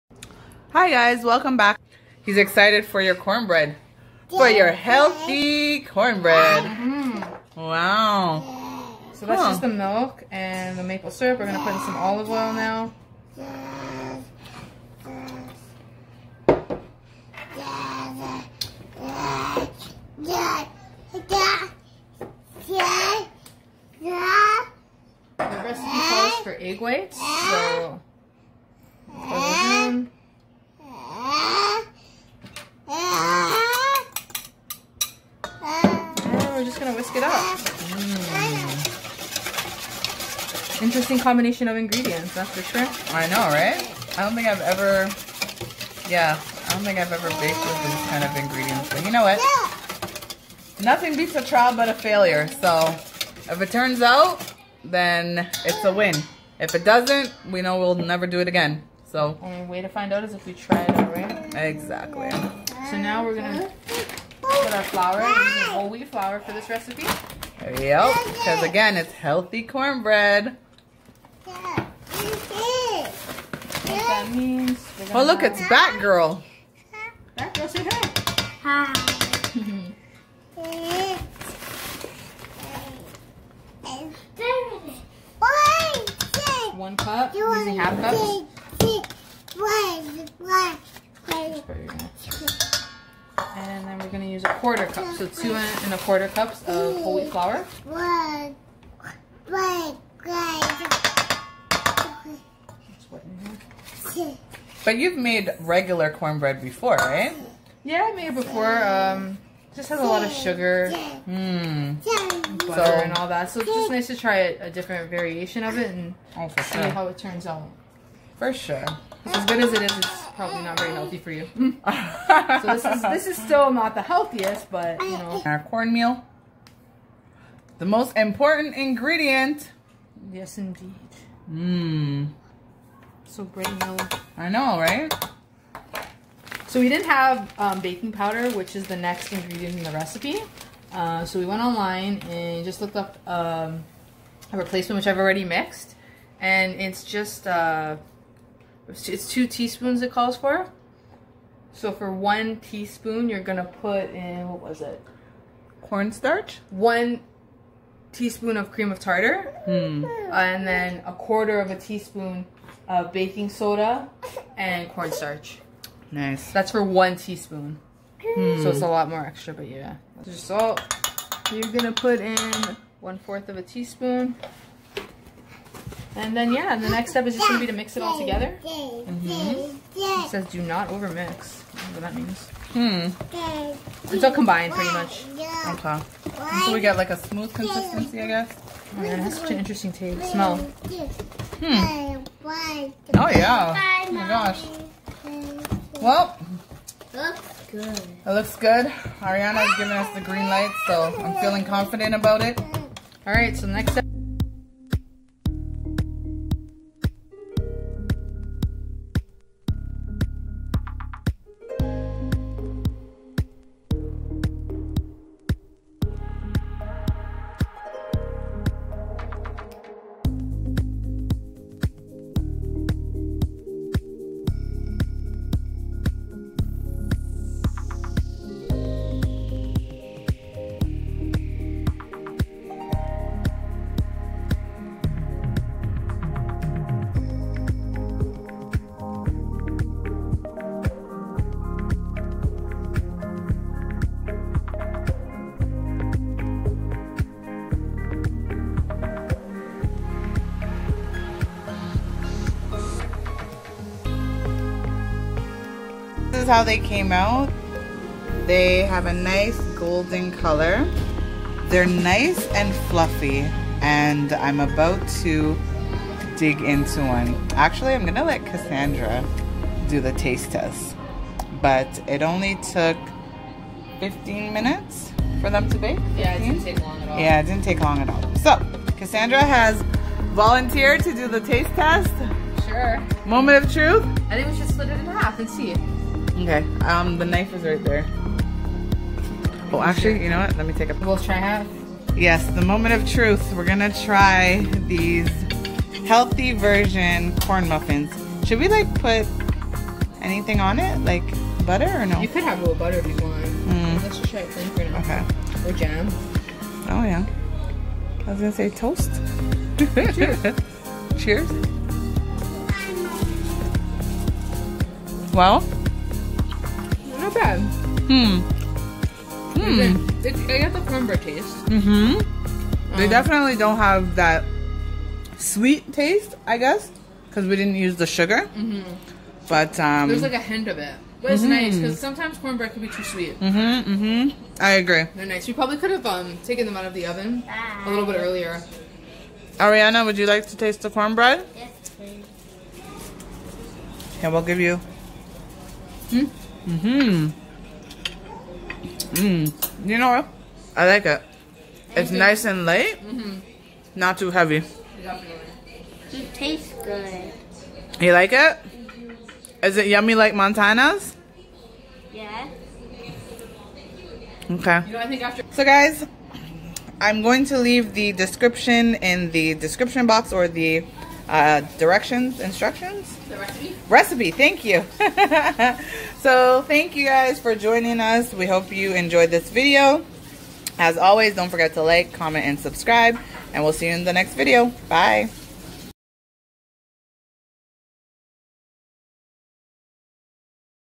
Easy Recipe | How To Make Fluffy Maple Cornbread
Hi Lov3's! Todaaaaay we are joined by a special guest...

Auntie Cass came to visit and it wouldn't be a visit with her if it didn't include baking🤗

She baked a healthier version of the traditional cornbread (which btw is my absolute favourite).